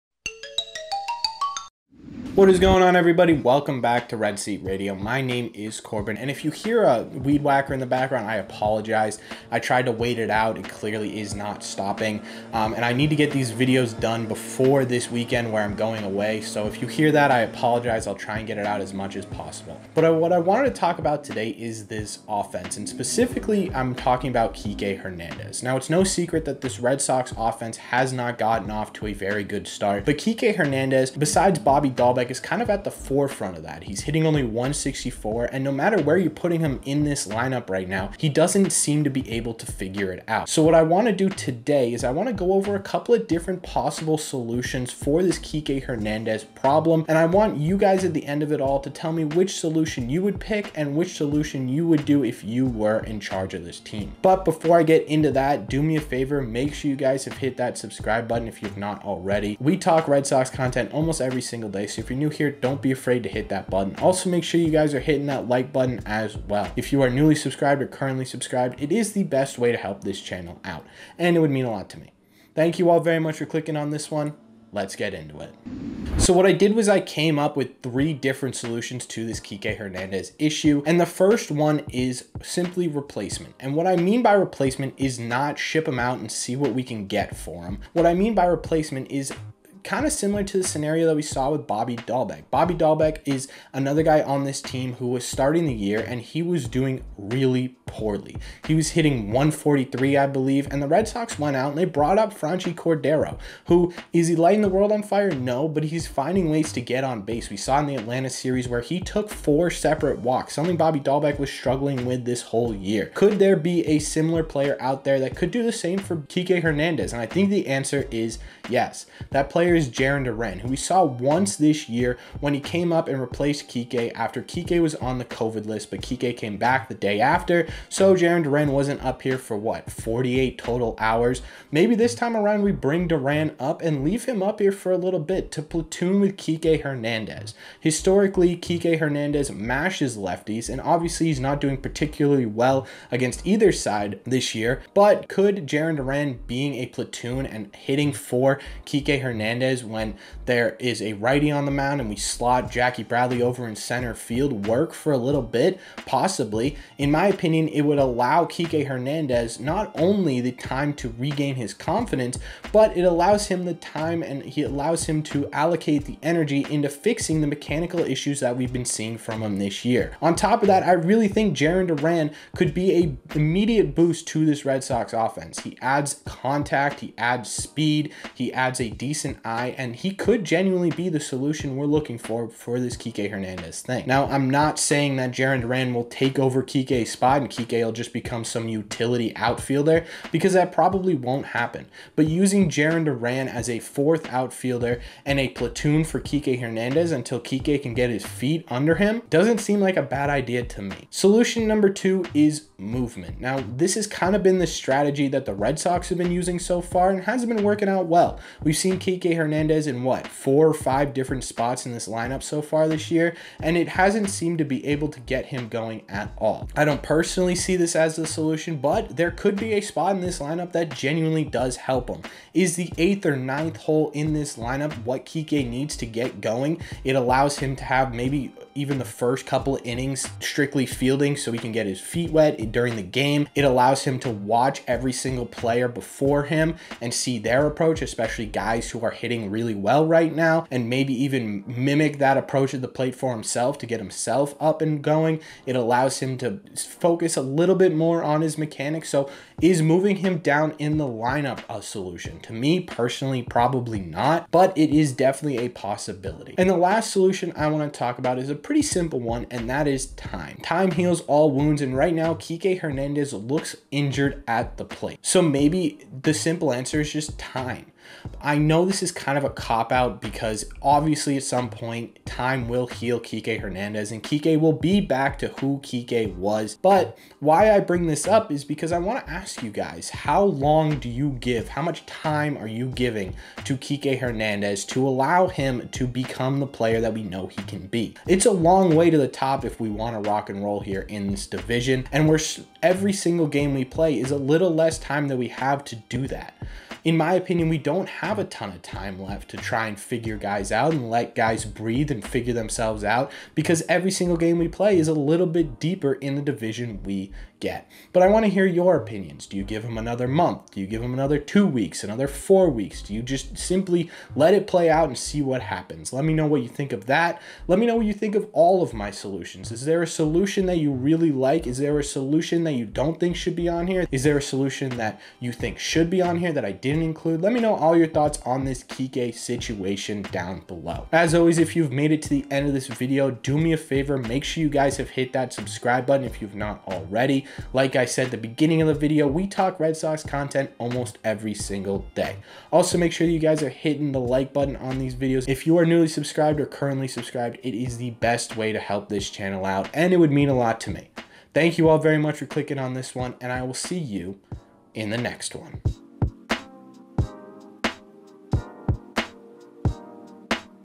Should the Red Sox REPLACE Kiké Hernandez??
Welcome back! It is no secret that the Red Sox offense is struggling. One guy right now who is kind of at the forefront of that is Kiké. In today's video we talk about possible solutions the Red Sox could possibly have for the lack of offense from their prolific center fielder. Including whether or not the Red Sox should think about replacing Kiké with someone else in this organization.